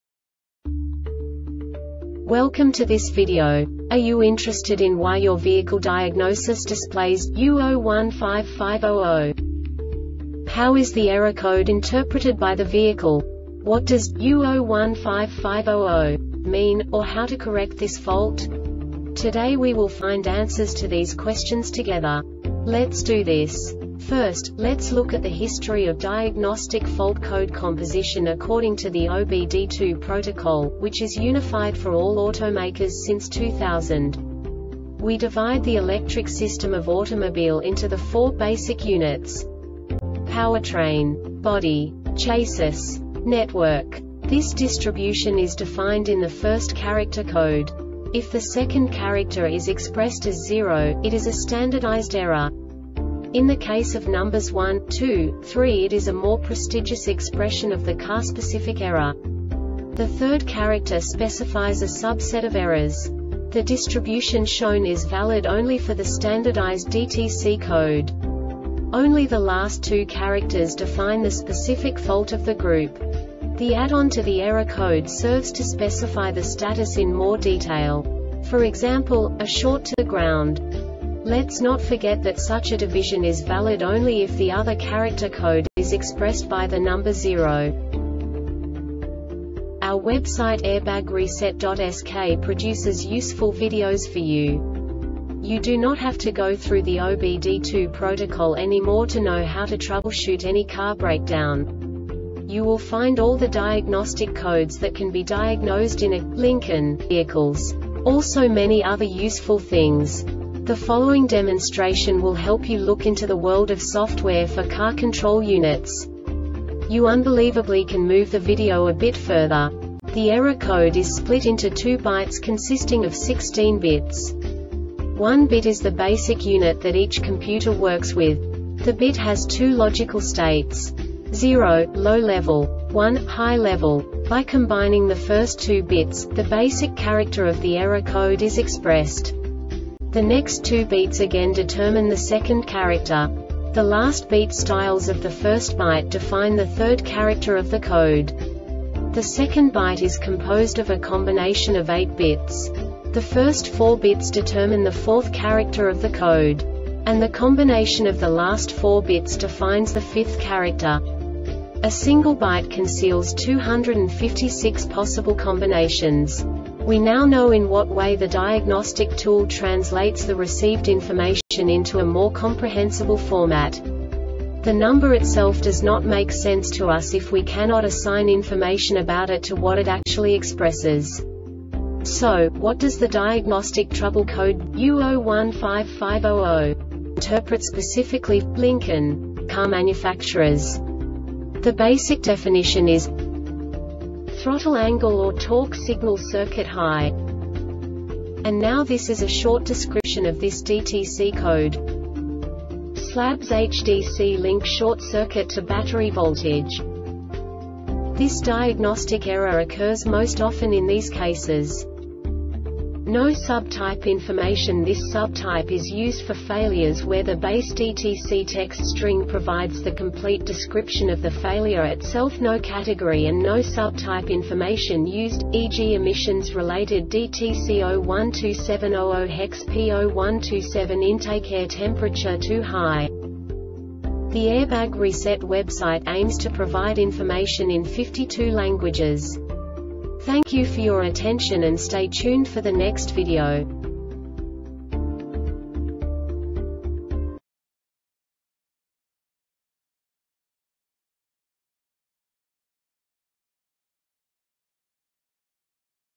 DTC Lincoln U0155-00 Short Explanation
Contents:
0:21 Basic DTC analysis according to OBD2 protocol standard.
1:48 Insight into programming
2:58 A diagnostically understandable description of the Diagnostic Trouble Code
3:05 Basic error definition

Make:
Lincoln
Code:
U0155 00
Definition:
Throttle angle or Torque signal circuit high
Description:
SLABS HDC link short circuit to battery voltage
Failure Type: